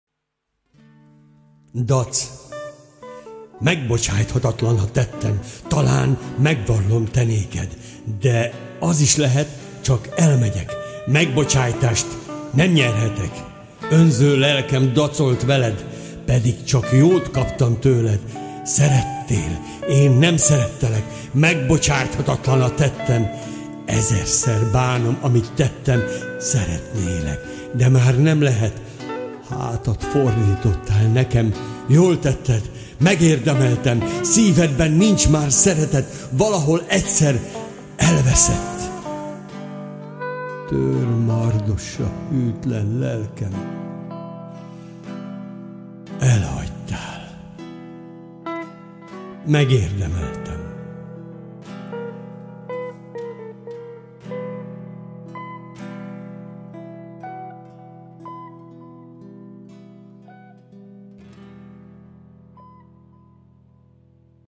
VARGA H MÁRIA DAC című verse, Elmondja: NÉMETH NYIBA SÁNDOR A MAGYAR KULTÚRA LOVAGJA
Vers, Irodalom, Németh Nyiba Sándor művészete, NYIBA MESTER, Olimpikon, Zenész